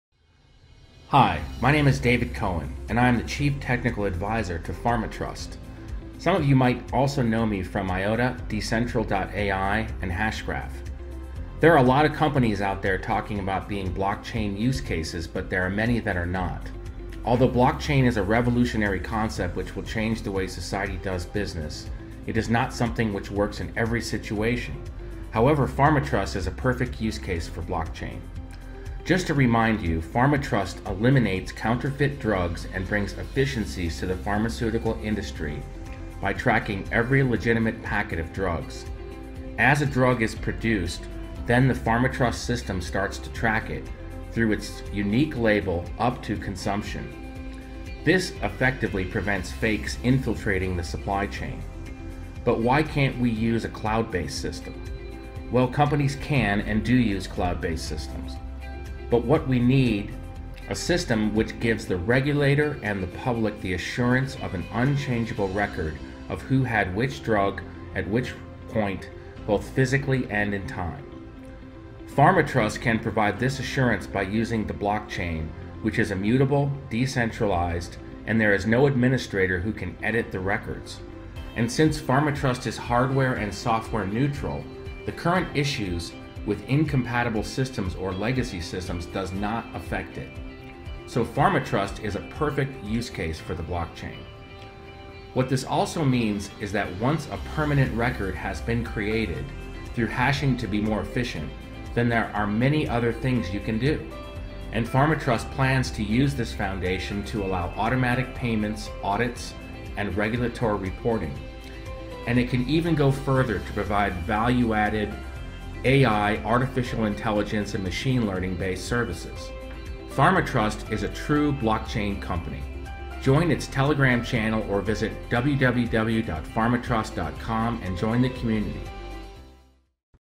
FarmaTrust Blockchain Use Case
Video setting out why FarmaTrust is the Perfect Blockchain Use Case